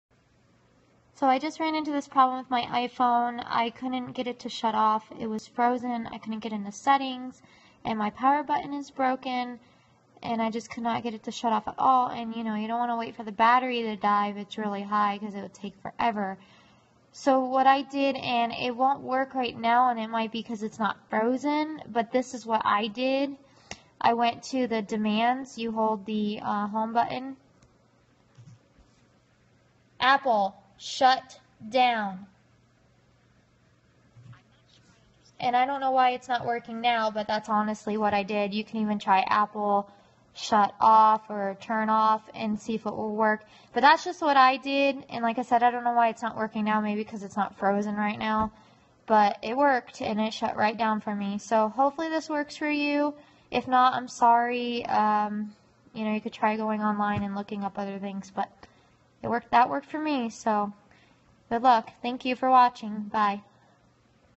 How to reset frozen Iphone when power button is broken
How to reset frozen Iphone when power button is broken. I hope it works for you!